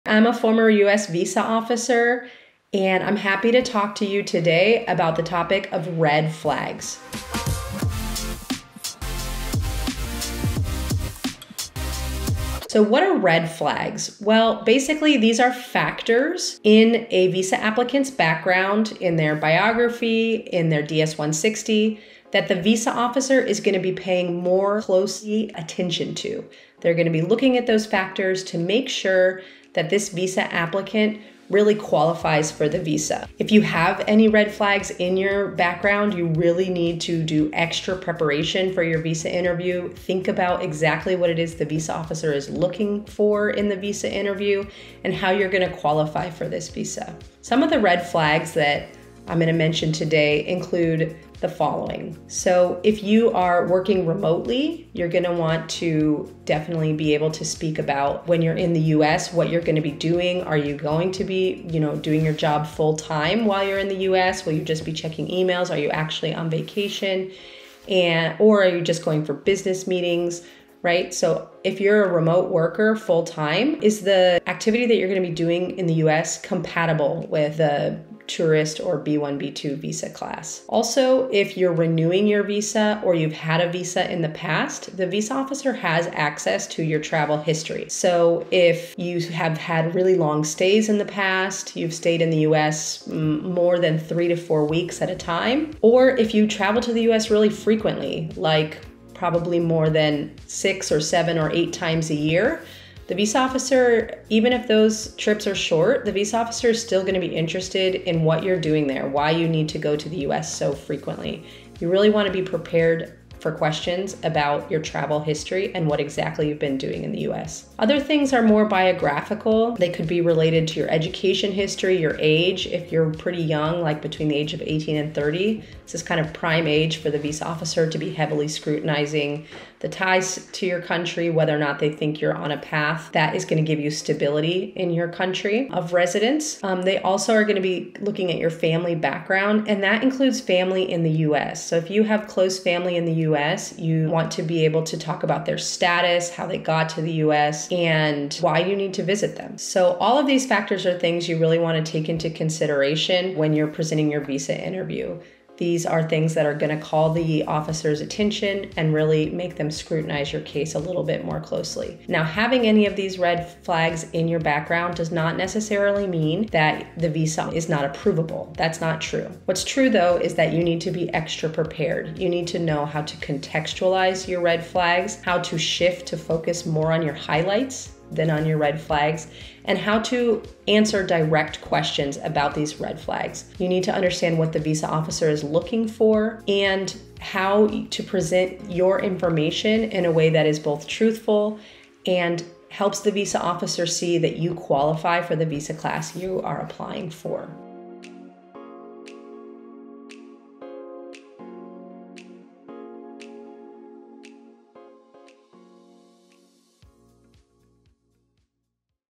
Visa Interview Red Flags: From the Perspective of a Former Visa Officer
Having red flags in your case does not mean that your visa can't be approved.

Having red flags does mean that you need to go in to your interview knowing what they are and have a plan to convince the officer why your visa should be approved. 💡

With Argo Visa you get the chance to work with the visa interview experts, our team of former visa officers.  👥 👥